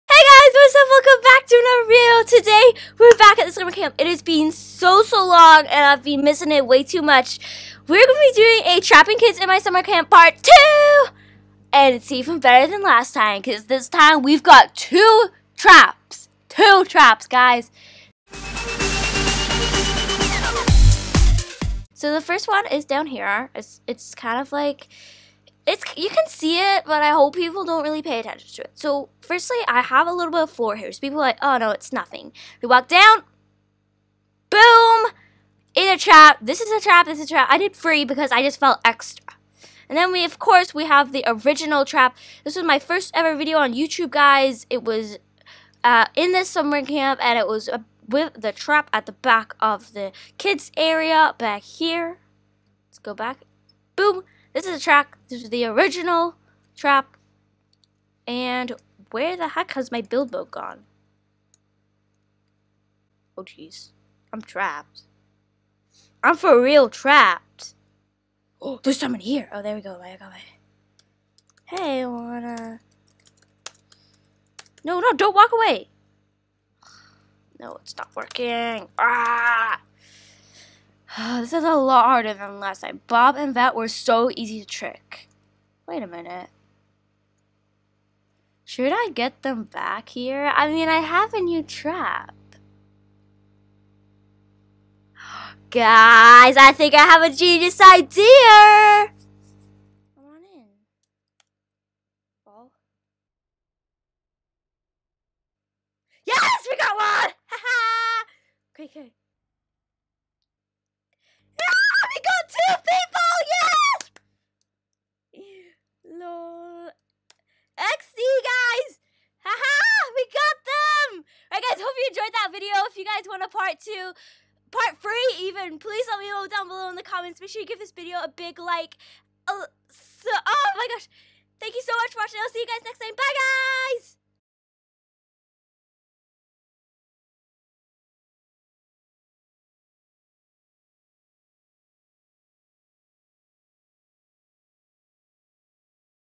TRAPPING KIDS IN MY SUMMER CAMP ~PART 2~
Hey! I saw you guys really liked the first trapping my kids in my summer camp video so I made a part 2. Would you like a part 3?? Maybe when I trap vet and bob?? If so comment